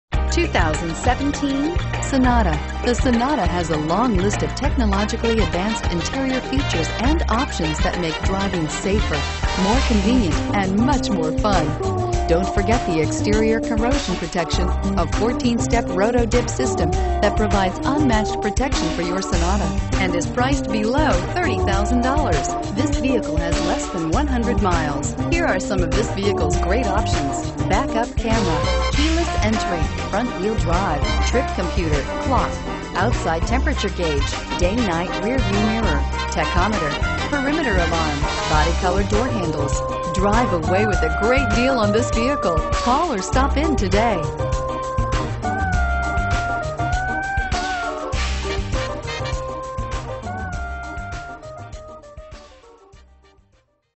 2017 Hyundai Sonata Puyallup WA H1064
2017 Hyundai Sonata Sport

2001 N. Meridian

Pick up this Pr 2017 Hyundai Sonata, available today at Larson Automotive Group. Featuring Automatic transmission, it has 9 miles. This could be the one you've been searching for.